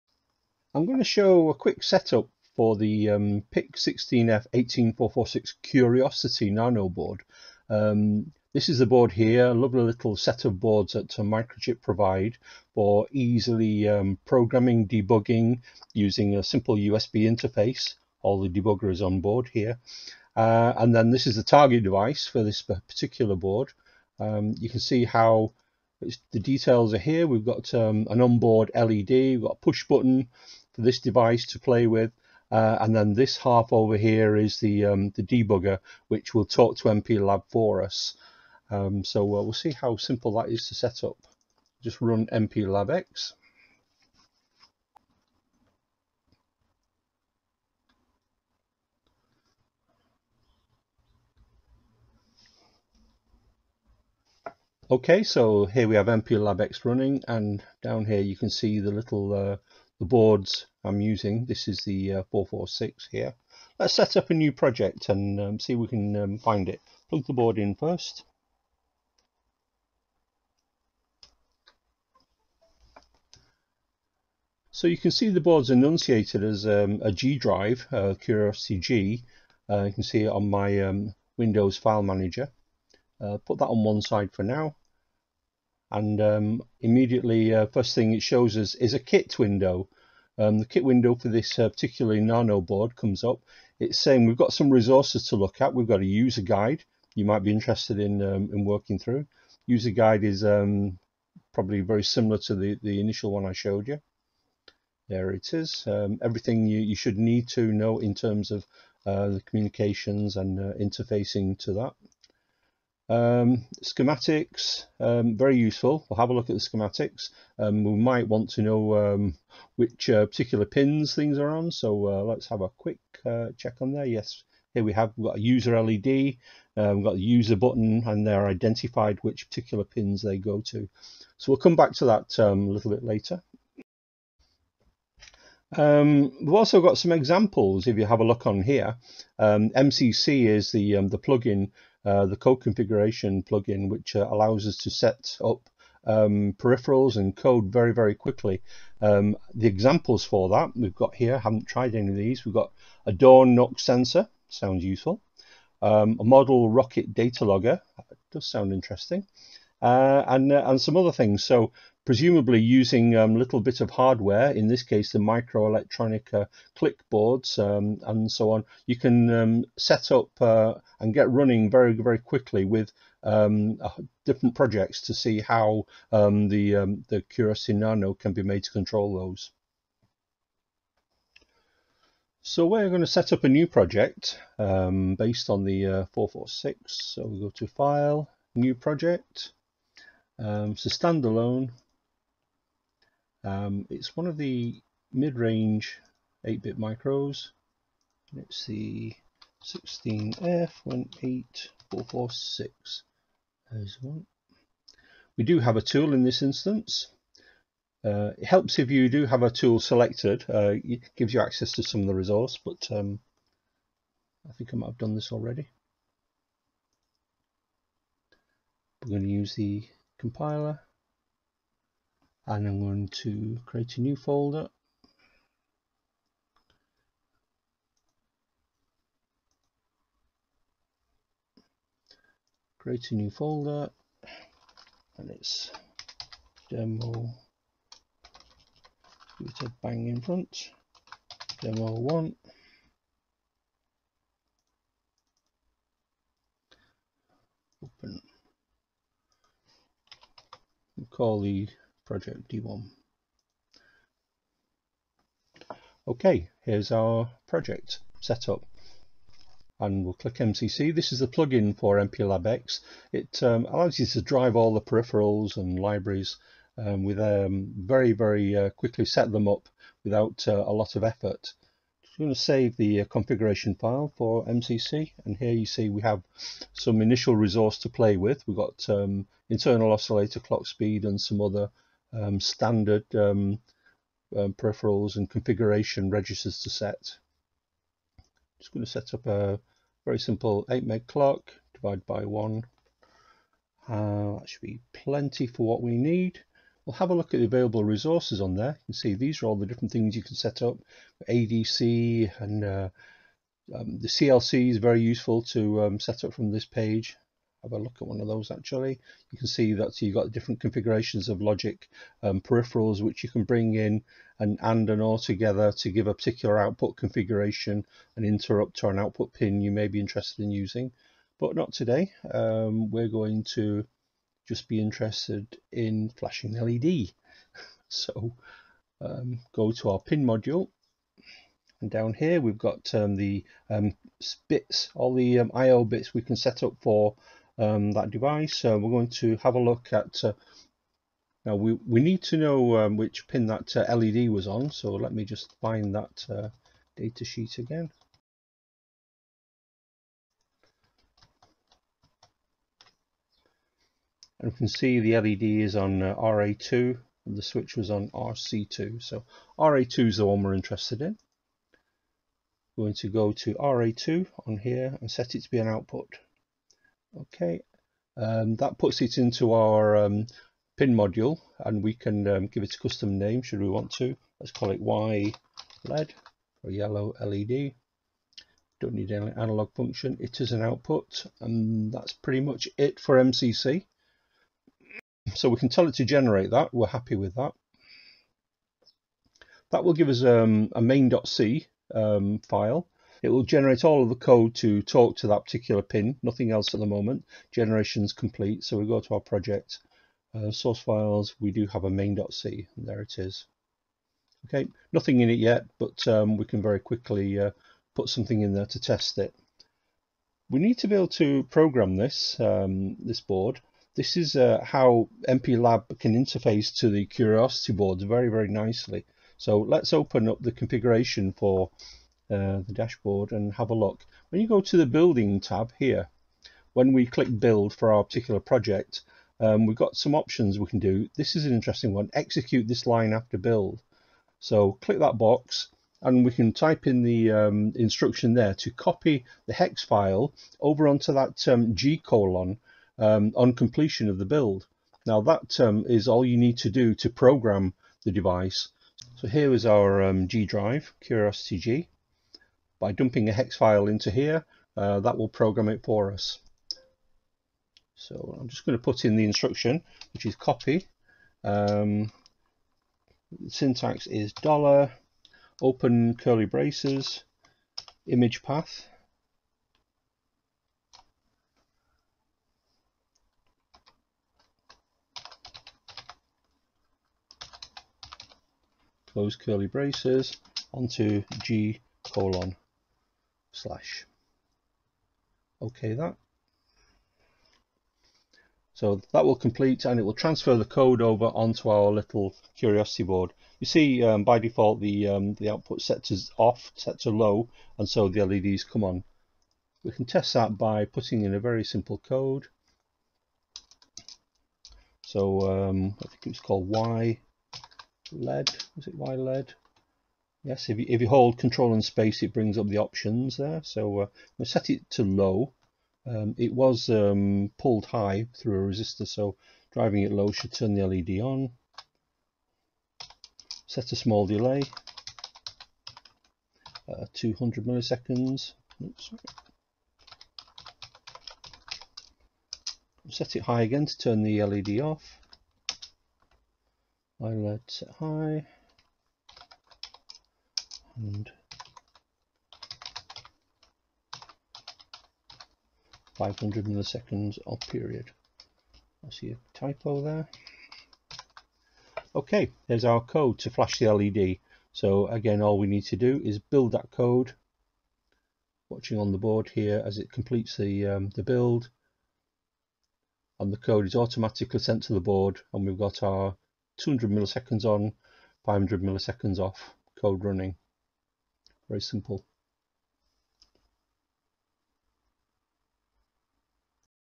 Using MPLAB-X to program Curiosity Boards
The Microchip Curiosity boards enable you to set up a project very quickly, and require no programmer or debugger - use this command to work easily with these modules.